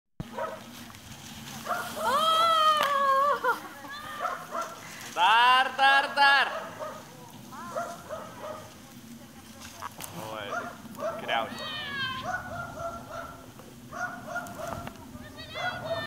P6270123
2015 birželio 27d.
Giminės susitikimas tėviškėje.
Vartų kaimas, Lazdijų rajonas LT